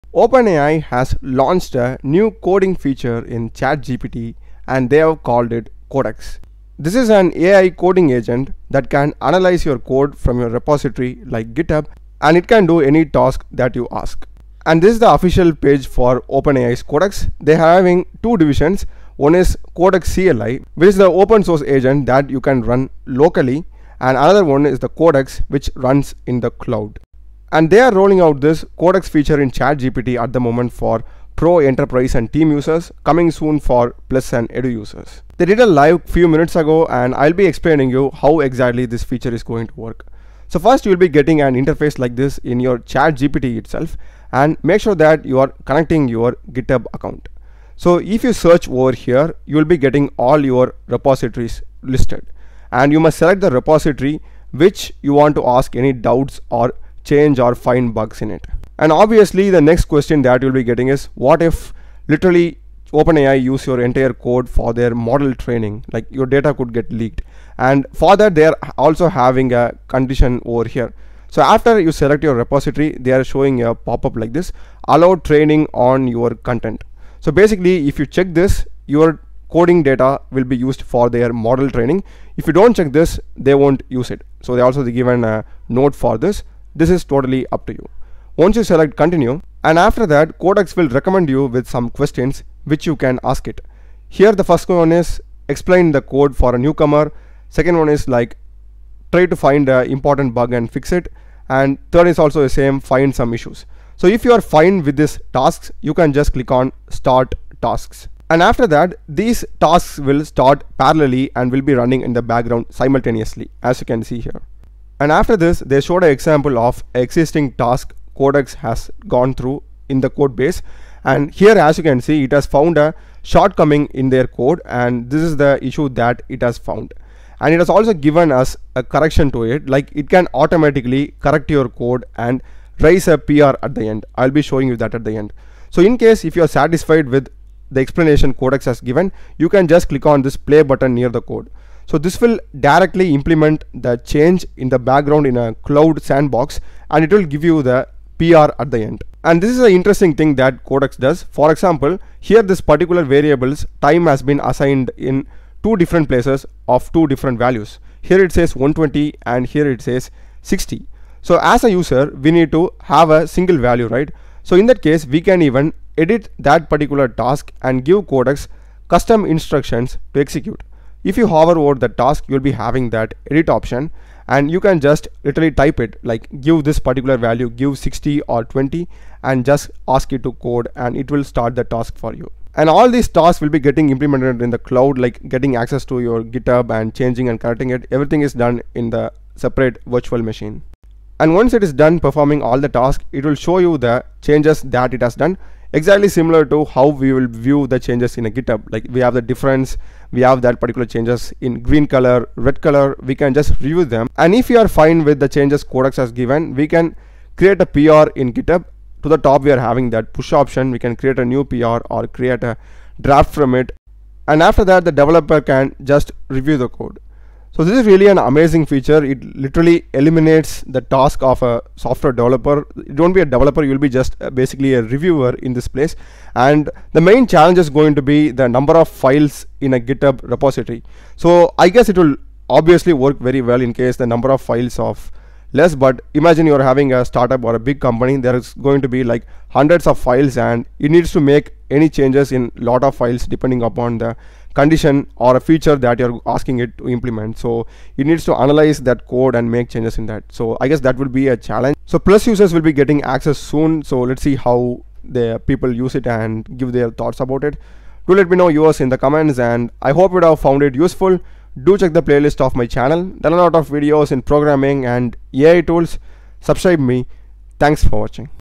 Codex in ChatGPT Explained - OpenAI's Coding Agent with GitHub
In this tutorial we will see how to use new openai's chatgpt update called codex which is a coding agent that can access your github repository code and perform tasks on your behalf